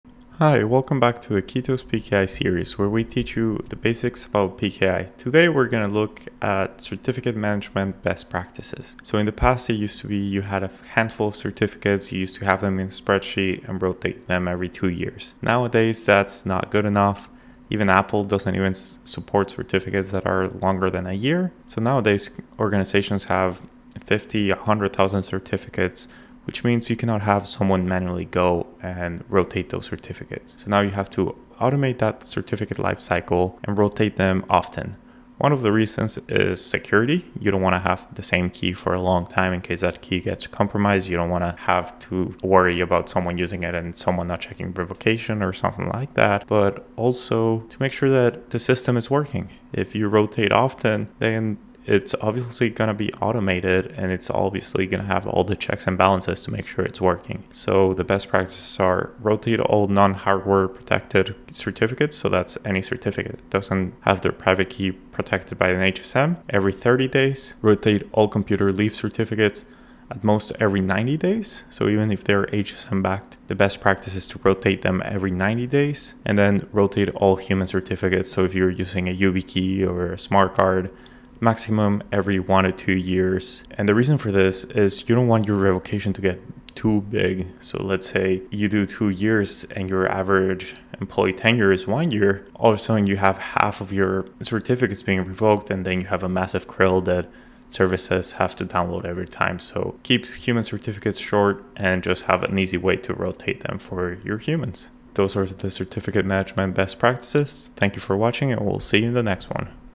X509 Certificate Management Best Practices
In this video, Keytos Founder Igal Flegmann will run through best practices for X509 Certificate Management.

Hi! Welcome back to the Keytos PKI series where we teach you the basics about PKI. Today we're going to look at certificate management best practices. In the past, it used to be you had a handful of certificates. You used to have them in spreadsheet and rotate them every two years. Nowadays, that's not good enough. Even Apple doesn't even support certificates that are longer than a year. So, nowadays organizations have 50-100,000 certificates, which means you cannot have someone manually go and rotate those certificates. So, now you have to automate that certificate lifecycle, and rotate them often. One of the reasons is security. You don't want to have the same key for a long time in case that key gets compromised. You don't want to have to worry about someone using it, and someone not checking revocation, or something like that. Also, to make sure that the system is working. If you rotate often, then it's obviously going to be automated, and it's obviously going to have all the checks and balances to make sure it's working. So, the best practices are rotate all non-hardware protected certificates. So, that's any certificate it doesn't have their private key protected by an HSM. Every 30 days, rotate all computer leave certificates, at most every 90 days. So, even if they’re HSM backed, the best practice is to rotate them every 90 days and then, rotate all human certificates. So, if you're using a YubiKey or a smart card, maximum every one to two years. The reason for this is you don't want your revocation to get too big. So, let's say you do two years, and your average employee tenure is 1 year, all of a sudden you have half of your certificates being revoked and then you have a massive Crl that services have to download every time. So, keep human certificates short, and just have an easy way to rotate them for your humans.